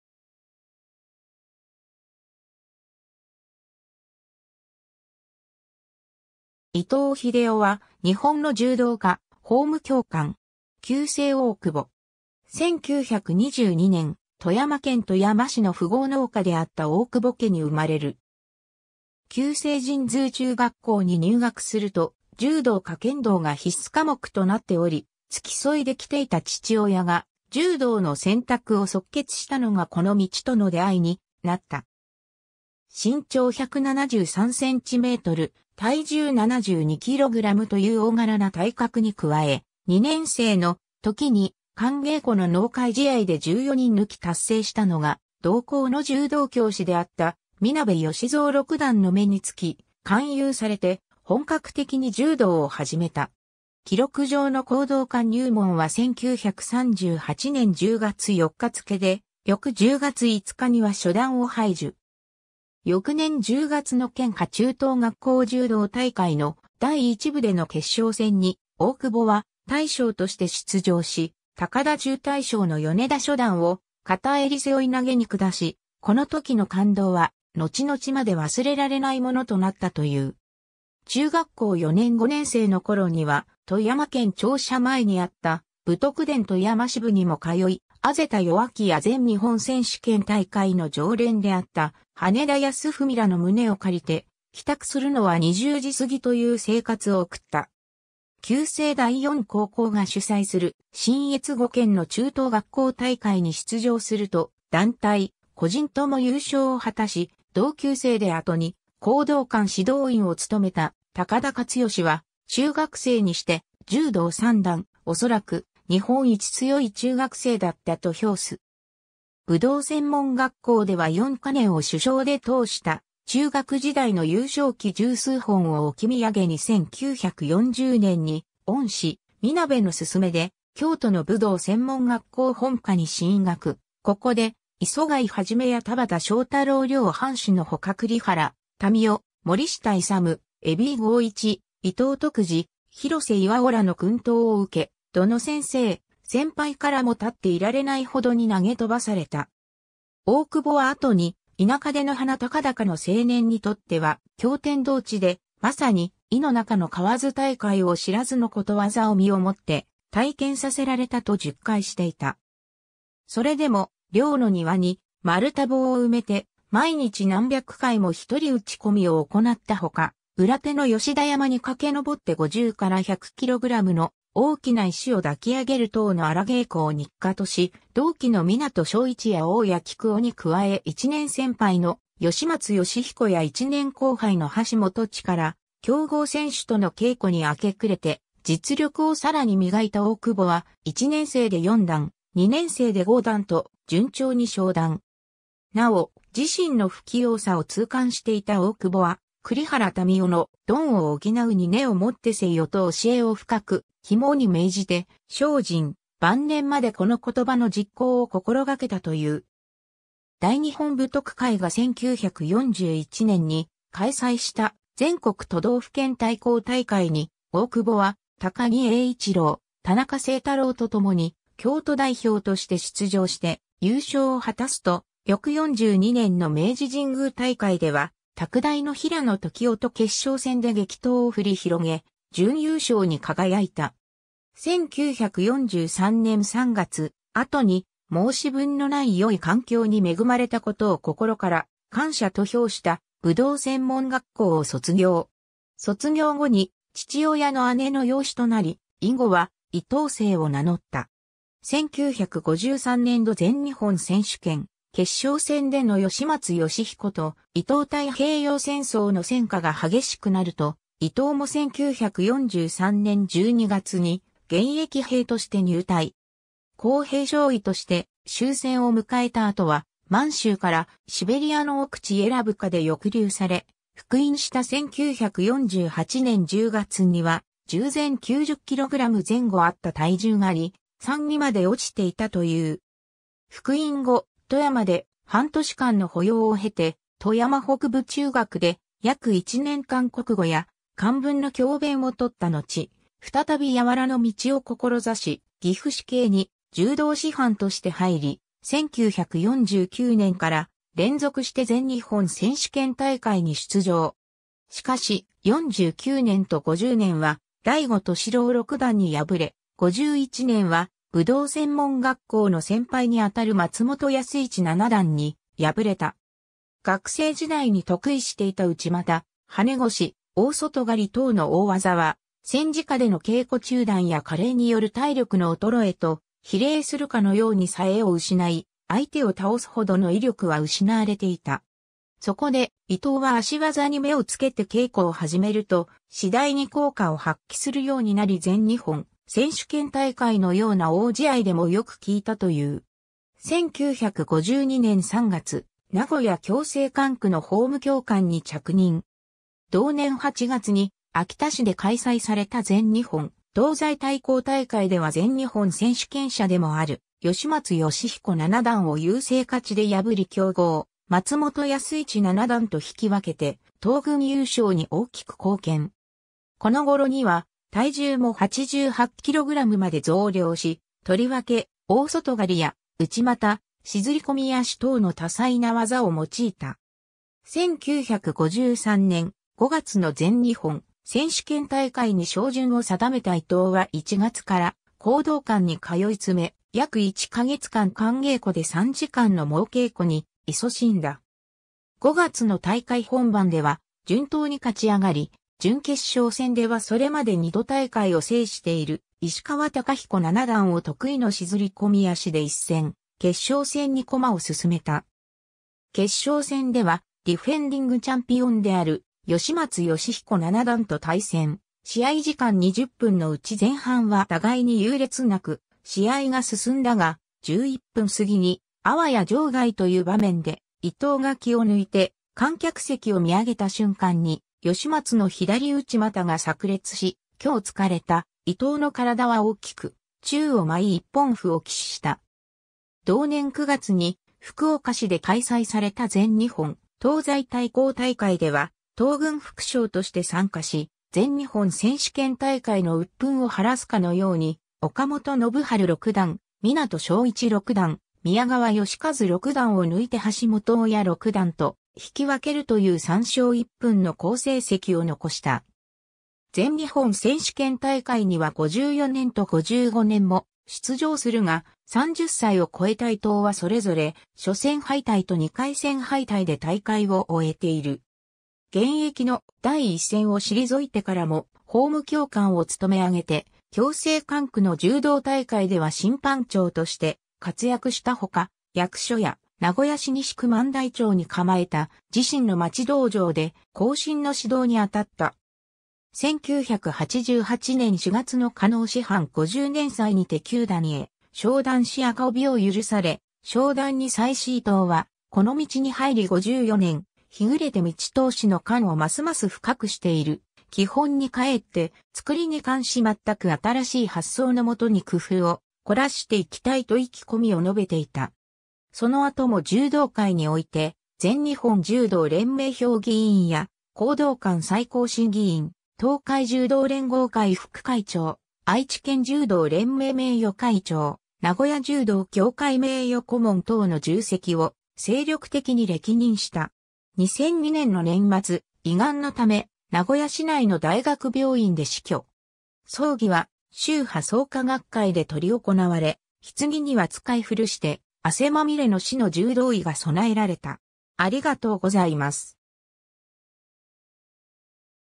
伊藤秀雄 (柔道)
伊藤 秀雄（いとう ひでお、1922年6月10日 - 2002年）は、日本の柔道家（講道館9段）、法務教官。
旧姓大久保。
 1922年、富山県富山市の富豪農家であった大久保家に生まれる。
旧制神通中学校（現・県立富山中部高校）に入学すると、柔道か剣道が必須科目となっており、付き添いで来ていた父親が柔道の選択を即決したのが斯の道との出会いになった。
身長173cm・体重72kgという大柄な体格に加え、2年生の時に寒稽古の納会試合で14人抜き達成したのが同校の柔道教師であった三鍋義三6段の目に付き、勧誘されて本格的に柔道を始めた。
記録上の講道館入門は1938年10月4日付で、翌10月5日には初段を拝受。
翌年10月の県下中等学校柔道大会の第一部での決勝戦（高田中学校戦）に大久保は大将として出場し、高田中大将の米田初段を片襟背負投に降し、この時の感動は後々まで忘れられないものとなったという。
 中学校4年・5年生の頃には富山県庁舎前にあった武徳殿富山支部にも通い、畔田与秋や全日本選士権大会の常連であった羽田泰文らの胸を借りて、帰宅するのは20時過ぎという生活を送った。
 旧制第四高校が主催する信越5県の中等学校大会に出場すると団体・個人とも優勝を果たし、同級生で後に講道館指導員を務めた高田勝善は「中学生にして柔道3段、恐らく日本一強い中学生だった」と評す。
 武道専門学校では4カ年を主将で通した 中学時代の優勝旗十数本を置き土産に1940年に恩師・三鍋の勧めで京都の武道専門学校本科に進学、ここで磯貝一や田畑昇太郎両範士のほか栗原民雄、森下勇、胡井剛一、伊藤徳治、広瀬巌らの薫陶を受け、どの先生・先輩からも立っていられない程に投げ飛ばされた。
大久保は後に「田舎出の鼻高々の青年にとっては驚天動地で、まさに“井の中の蛙大海を知らず”の諺を身を以って体験させられた」と述懐していた。
 それでも、寮の庭に丸太棒を埋めて毎日何百回も1人打ち込みを行ったほか、裏手の吉田山に駆け上って50~100kgの大きな石を抱き上げる等の荒稽古を日課とし、同期の湊庄市や大矢喜久雄に加え1年先輩の吉松義彦や1年後輩の橋元親ら強豪選手との稽古に明け暮れて実力を更に磨いた大久保は、1年生で4段、2年生で5段と順調に昇段。
 なお、自身の不器用さを痛感していた大久保は、栗原民雄の「鈍を補うに根をもってせよ」と訓えを深く肝に銘じて精進、晩年までこの言葉の実行を心掛けたという。
 大日本武徳会が1941年に開催した全国都道府県対抗大会（団体戦）に大久保は高木栄一郎・田中清太郎と共に京都代表として出場して優勝を果たすと、翌42年の明治神宮大会では拓大の平野時男と決勝戦で激闘を振り広げ準優勝に輝いた。
1943年3月、後に「申し分のない良い環境に恵まれた事を心から感謝」と評した武道専門学校を卒業。
卒業後に父親の姉の養子となり、以後は伊藤姓を名乗った。
 1953年度全日本選手権決勝戦での吉松義彦（左）と伊藤（右） 太平洋戦争の戦禍が激しくなると、伊藤も1943年12月に現役兵として入隊。
工兵少尉として終戦を迎えた後は満州からシベリアの奥地エラブカで抑留され、復員した1948年10月には、従前90kg前後あった体重が2/3にまで落ちていたという。
 復員後、富山で半年間の保養を経て富山北部中学で約1年間国語や漢文の教鞭を執ったのち、再び柔の道を志し岐阜市警に柔道師範として入り、1949年から連続して全日本選手権大会に出場。
しかし、49年と50年は醍醐敏郎6段に敗れ、51年は武道専門学校の先輩に当たる松本安市7段に敗れた。
学生時代に得意していた内股・跳腰・大外刈等の大技は、戦時下での稽古中断や加齢による体力の衰えと比例するかのように冴えを失い、相手を倒す程の威力は失われていた。
そこで伊藤は足技に目を付けて稽古を始めると、次第に効果を発揮するようになり全日本選手権大会のような大試合でもよく効いたという。
 1952年3月、名古屋矯正管区の法務教官に着任。
同年8月に秋田市で開催された全日本東西対抗大会では全日本選手権者でもある吉松義彦7段を優勢勝で破り強豪・松本安市7段と引き分けて、東軍優勝に大きく貢献。
この頃には体重も88kgまで増量し、とりわけ大外刈や内股、支釣込足等の多彩...